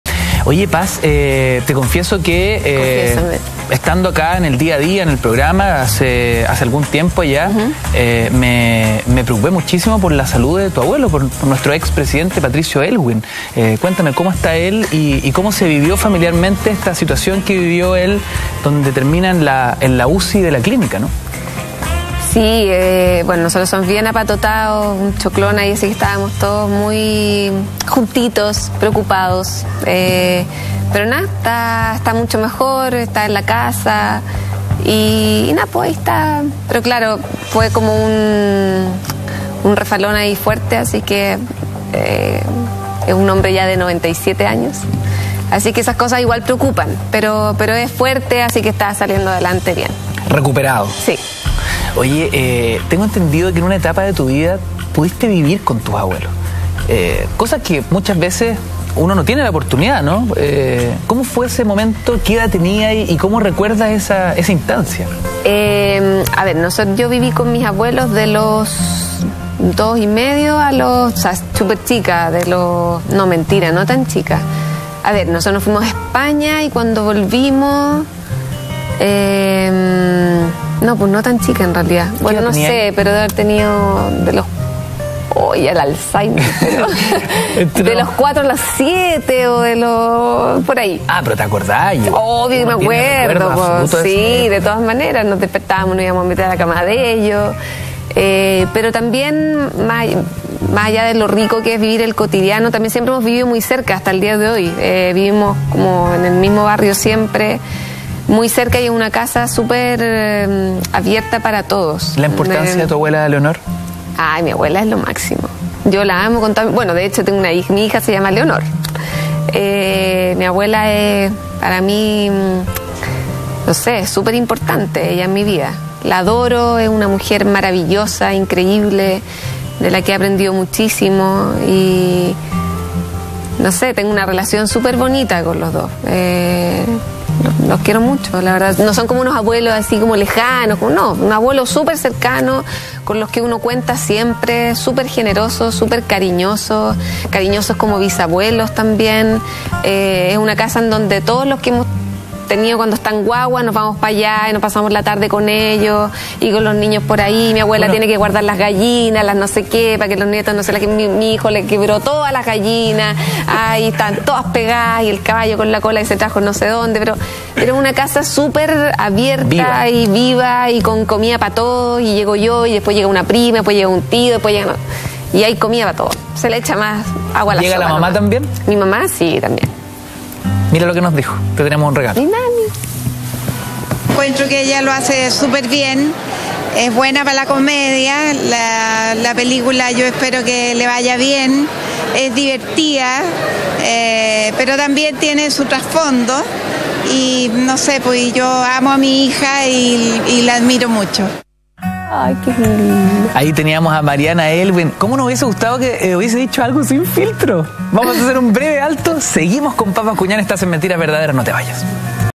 La relación de Paz Bascuñán con su abuelo Patricio Alwyn
La actriz nos comentó de su cercana relación con el ex Presidente de la República.